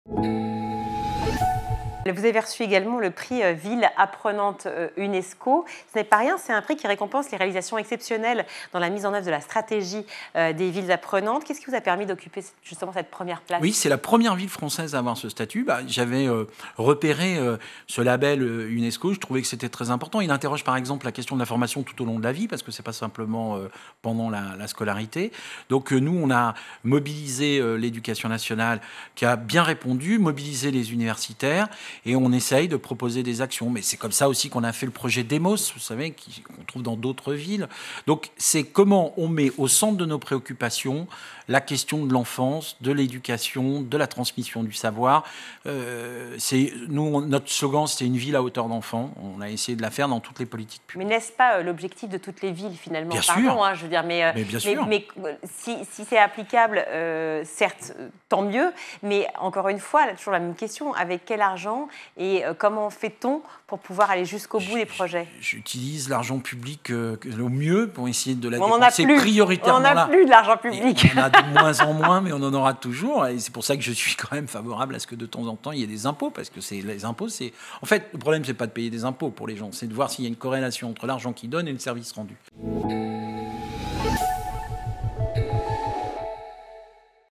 Clermont-Ferrand: première "ville apprenante" labellisée UNESCO !
"C'est la première ville française à obtenir ce statut". Ce label interroge sur la question de la formation tout au long de la vie. Nous avons mobilisé l'Éducation nationale et les universitaires. On essaie de proposer des actions. Notre slogan : "une ville à hauteur d'enfants!" insiste Olivier Bianchi, maire de Clermont-Ferrand et président de Clermont Ferrand Auvergne Métropole. Il revient sur l'objectif de ce label : récompenser la ville labellisée, pour garantir une éducation de qualité, inclusive et équitable.